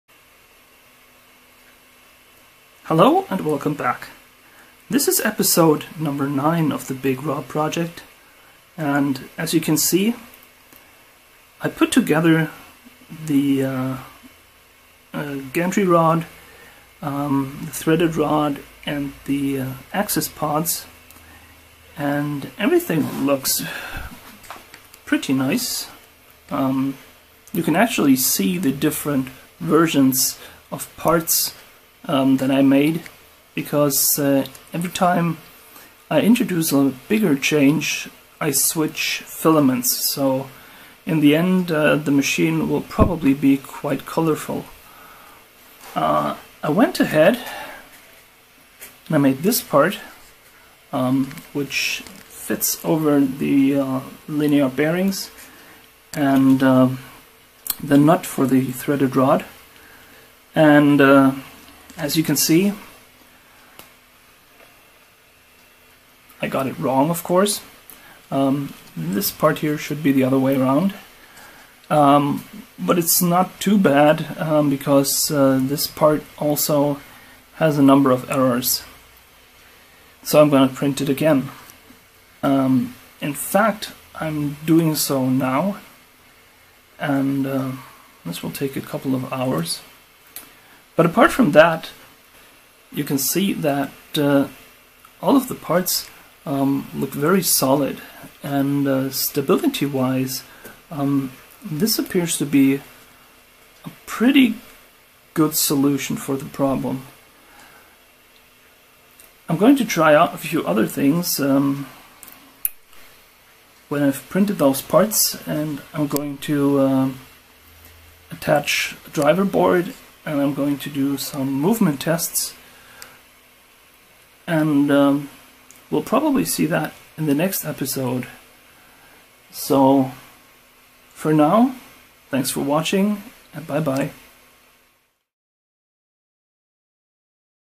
Big Rob Project Part 9
The Big Rob Project is the attempt to build a large CNC table
with X,Y, and Z Axis. It is intended to be multifunctional.

I have no Idea where this will go, I'm just documenting the journey.